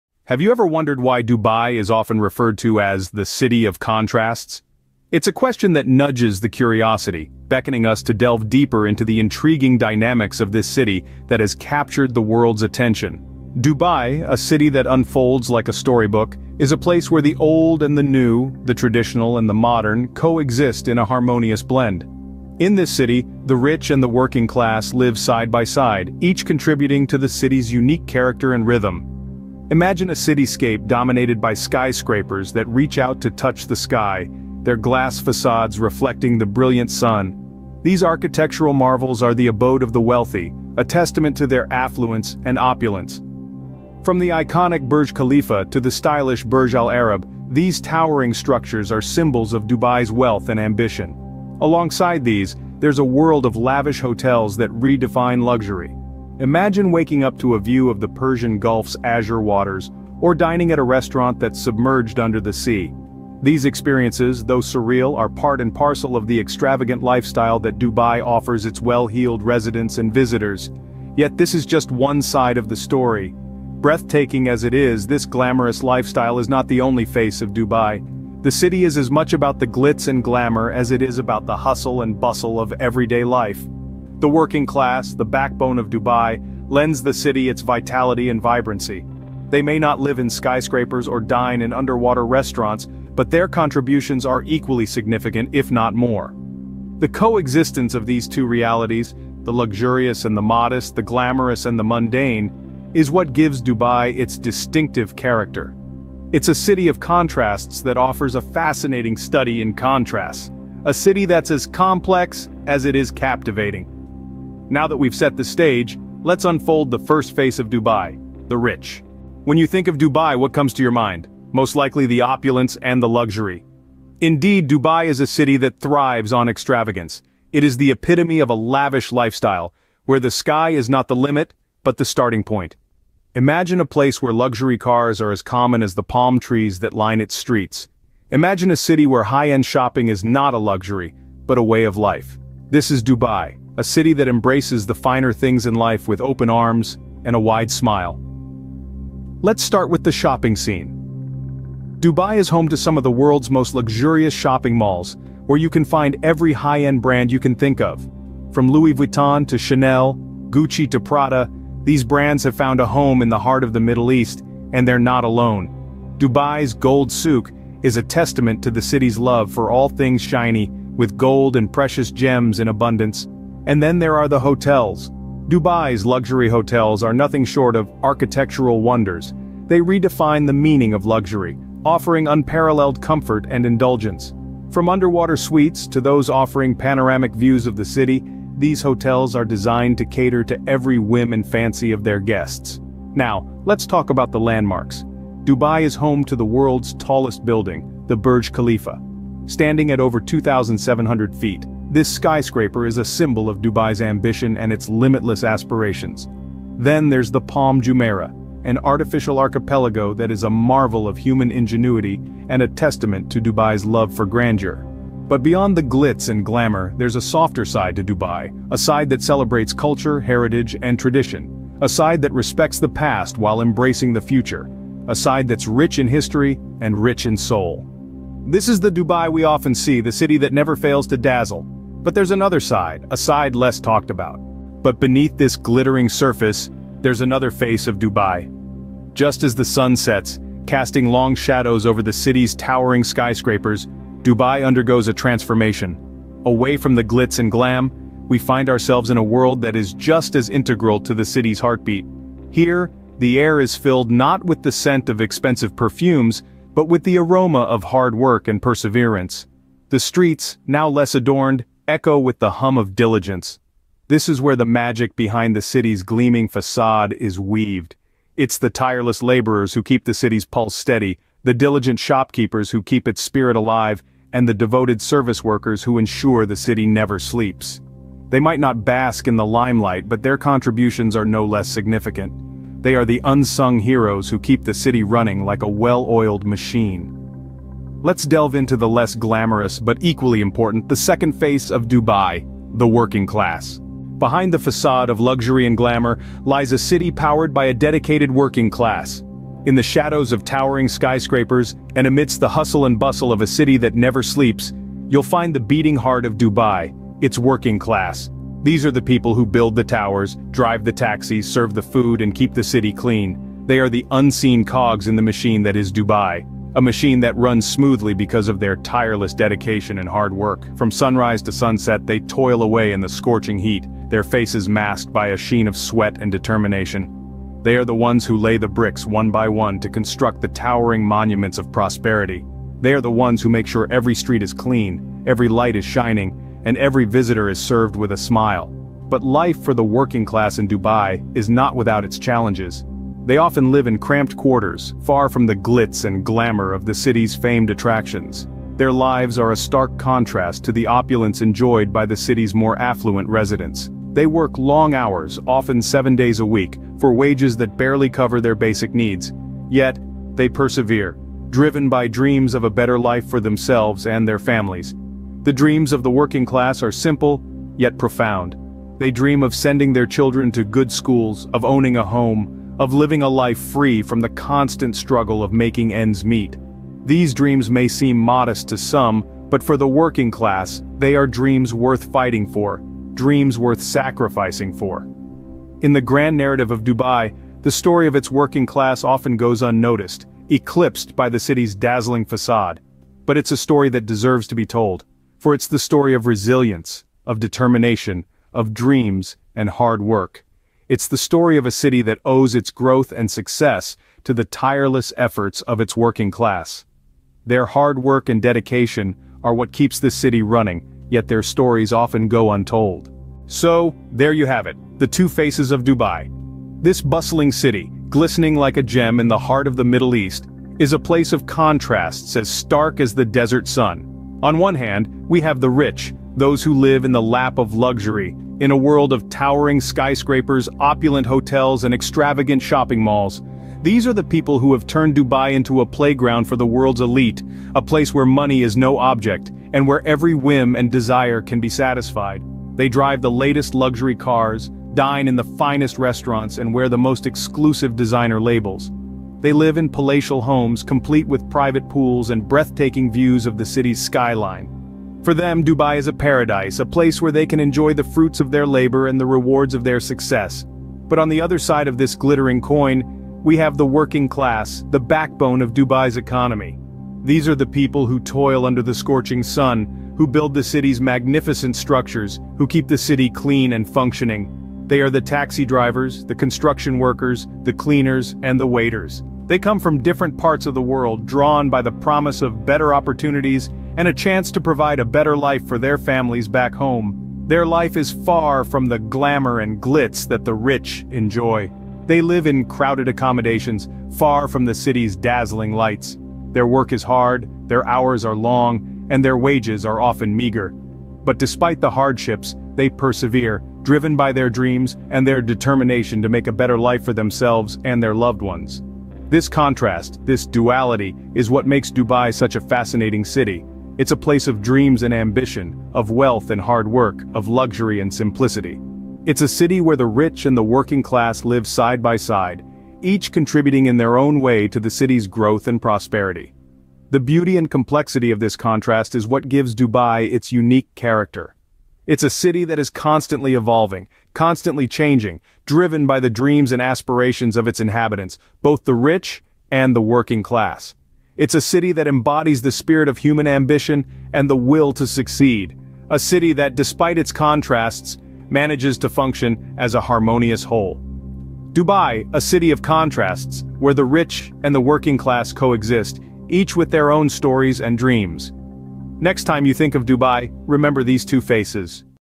Two Faces Of Dubai: The Rich And The Working Class
In this captivating YouTube video, we delve into the stark contrast between the two faces of Dubai - the affluent and the working class. Explore the opulent lifestyle of the rich, with their luxurious mansions, extravagant cars, and lavish shopping experiences. Witness the glitz and glamour that Dubai is renowned for. On the other hand, gain insight into the lives of the hardworking individuals who contribute to the city's growth and development. Discover their daily struggles, aspirations, and the challenges they face. This thought-provoking video sheds light on the disparities that exist within this vibrant city, offering a unique perspective on Dubai's social fabric.